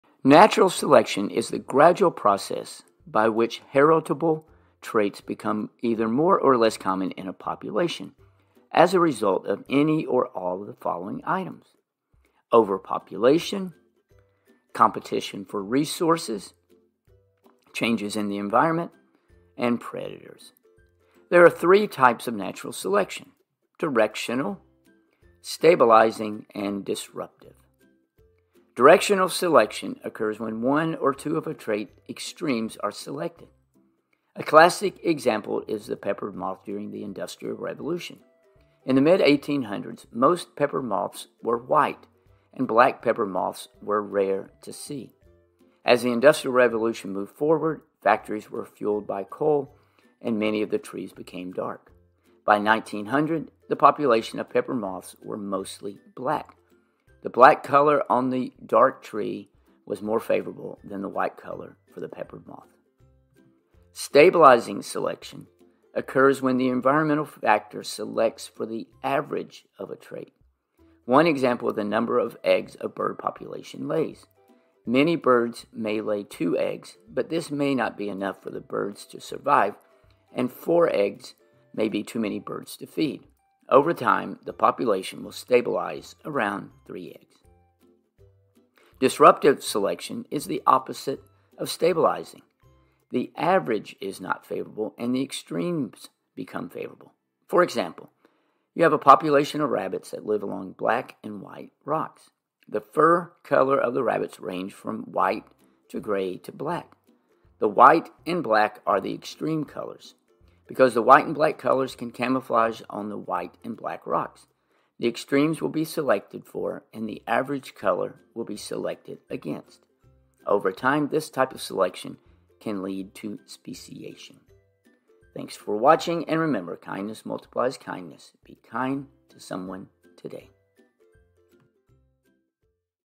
Natural Selection | 3 Types
In this video, I cover three types of natural selection. Directional
Stabilizing and Disruptive.
Directional selection involves the extremes being selected. A classic example is the peppered moth.
Stabilizing selection involves the average being the best choice.
Disruptive involves chooses multiple extremes and can lead to speciation.